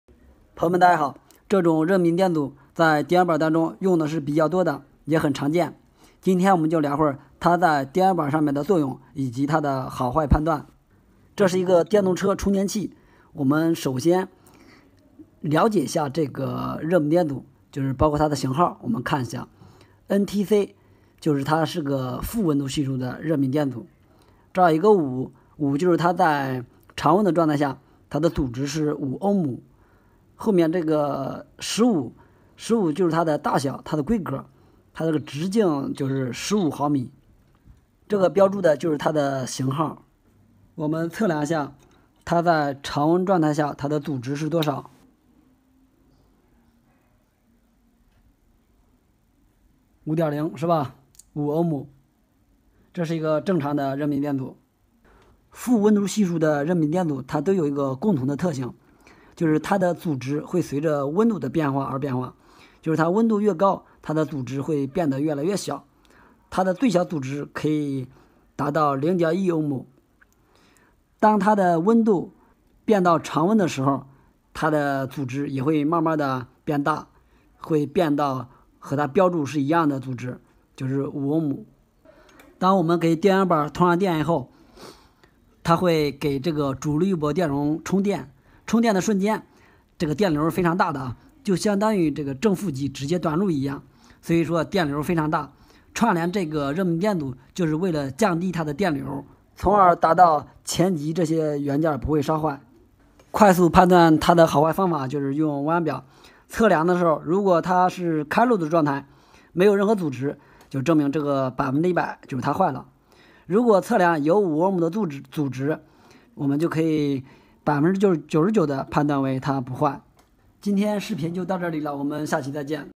NTC热敏电阻有什么作用？如何快速判断好坏？方法简单实用#我爱电器维修
我们的视频是为了为观众提供有价值的内容，绝对没有任何违规行为。 NTC热敏电阻有什么作用？如何快速判断好坏？方法简单实用

非专业人员请勿直接操作！